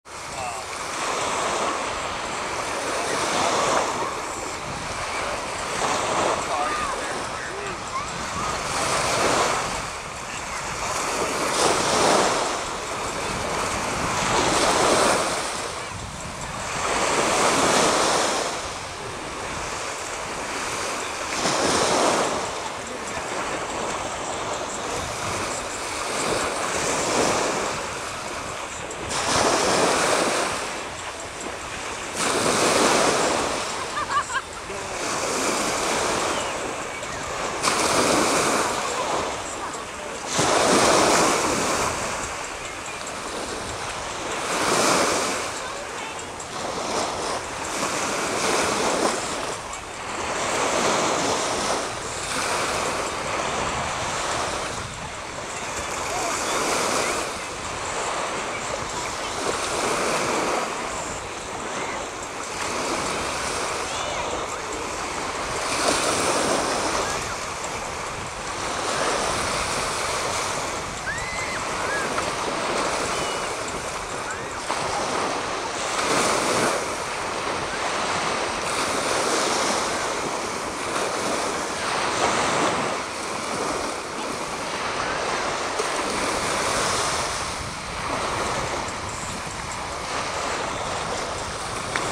meiko the very happy dog
This sweet little dog loves the Chesapeake Bay.  She's so happy out there.